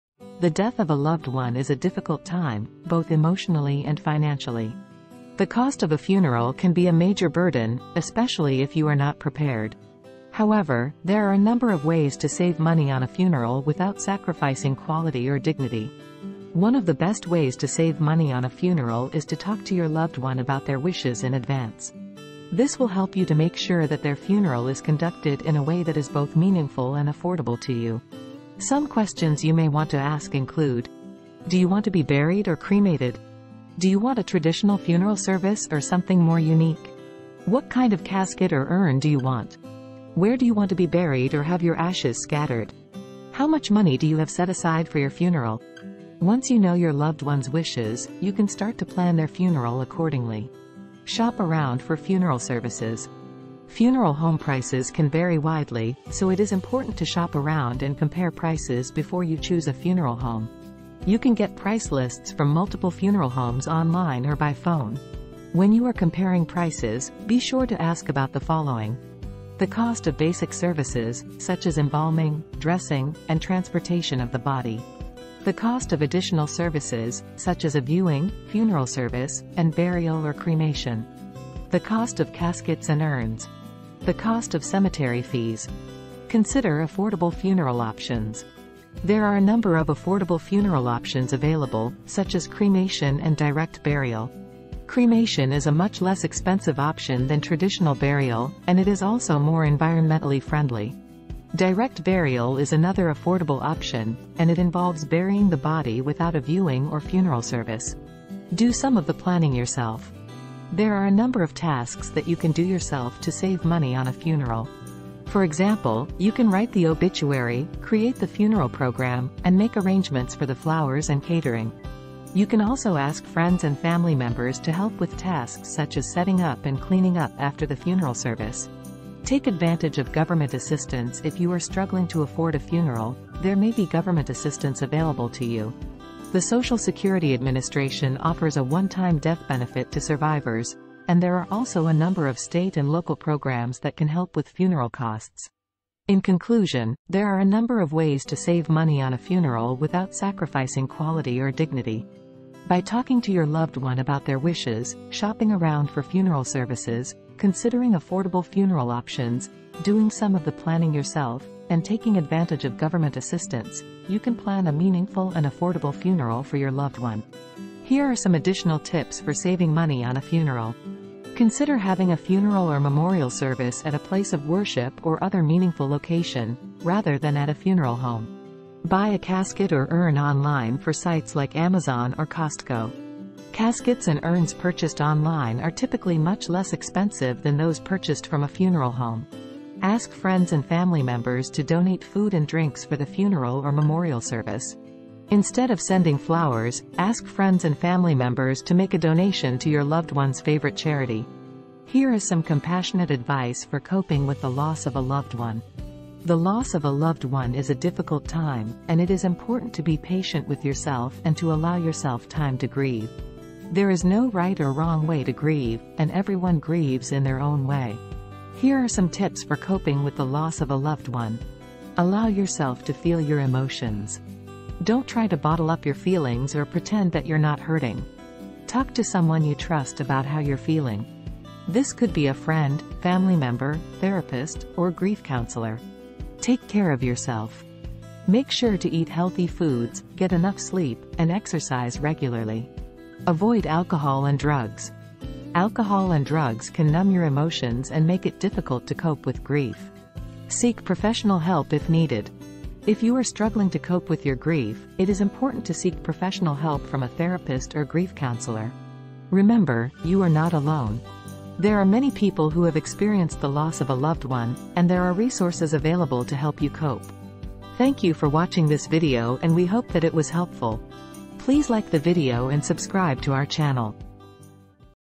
How to Save Money on a Funeral
The death of a loved one is a difficult time, both emotionally and financially. The cost of a funeral can be a major burden, especially if you are not prepared. However, there are a number of ways to save money on a funeral without sacrificing quality or dignity.

for more information.